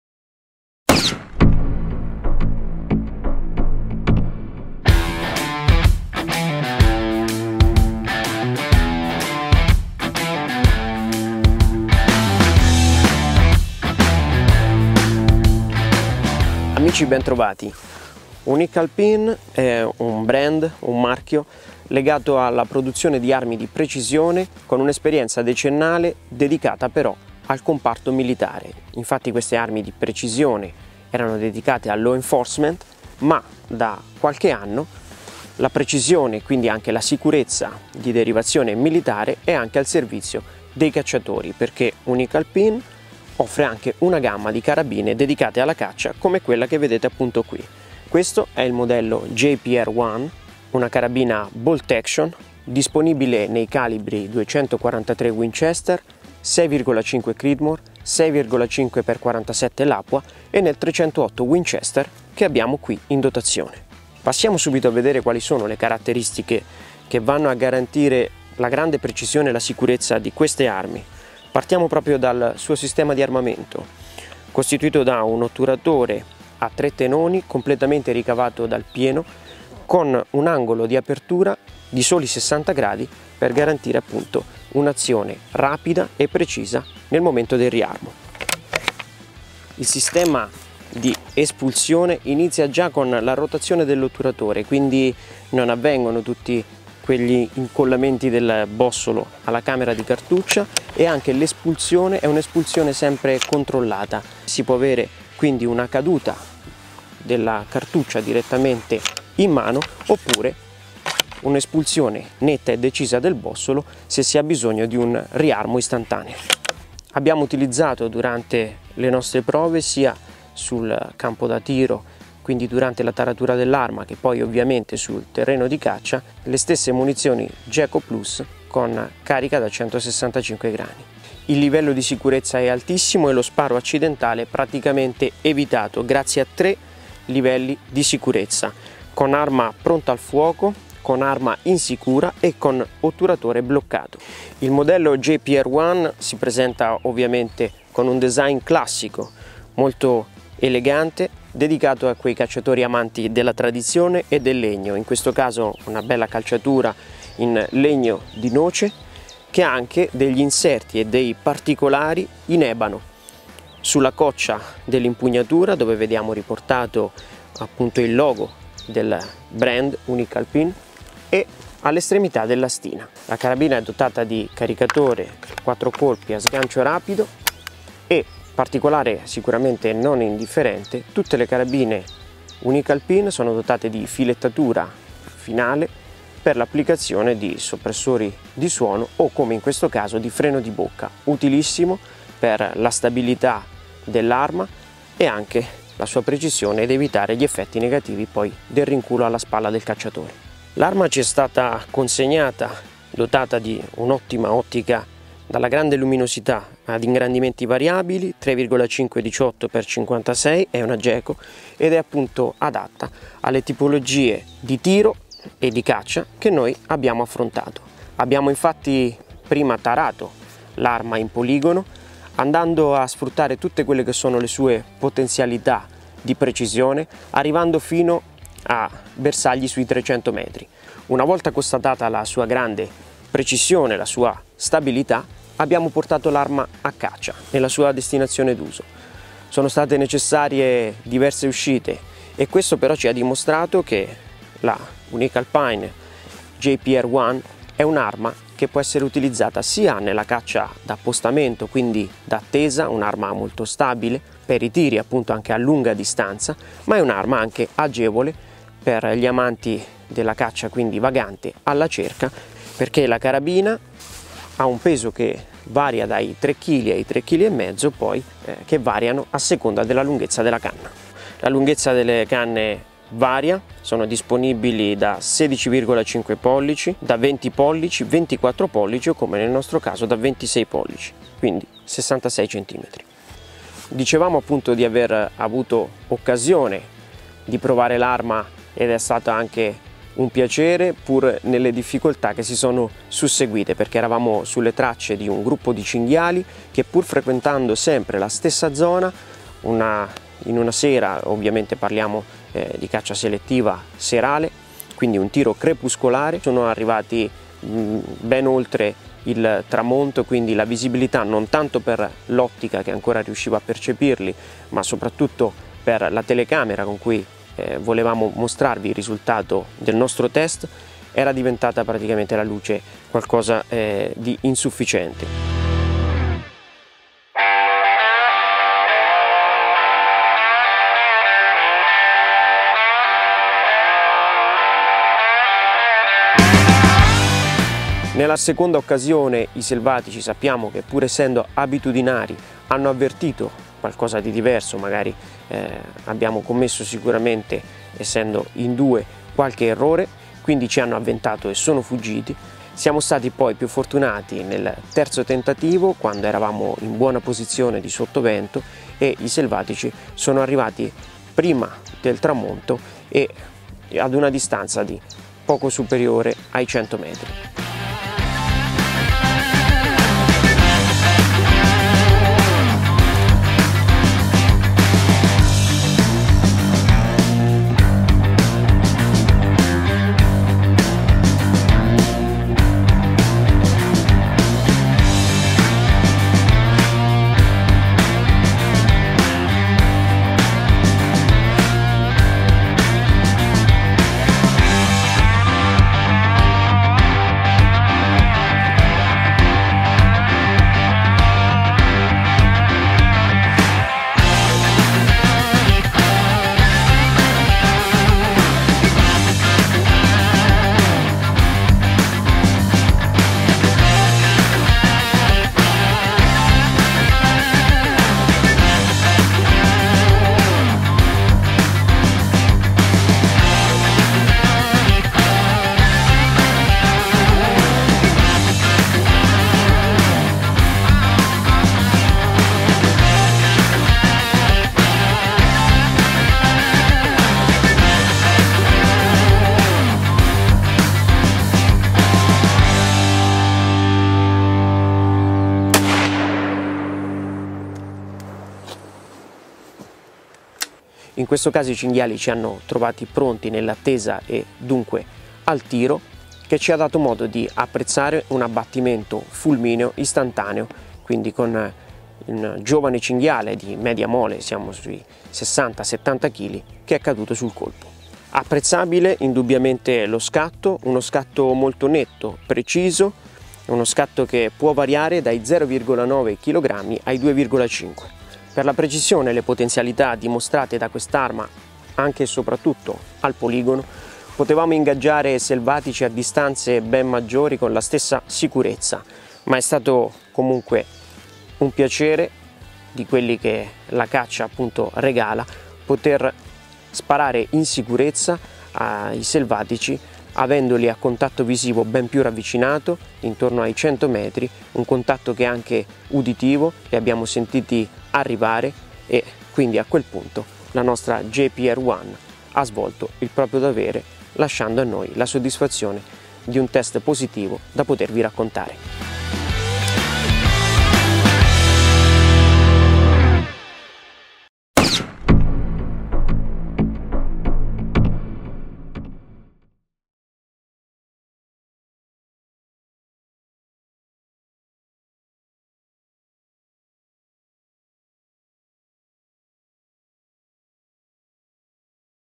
Carabina bolt action JPR 1 di Unique Alpine. Caccia al cinghiale, boar hunting.
Una gamma di carabine dedicate alla caccia dalla precisione e sicurezza che derivano da decenni di esperienza nel settore militare. Parliamo delle bolt action Unique Alpine di cui abbiamo provato in poligono e a caccia il modello JPR 1 che vi raccontiamo.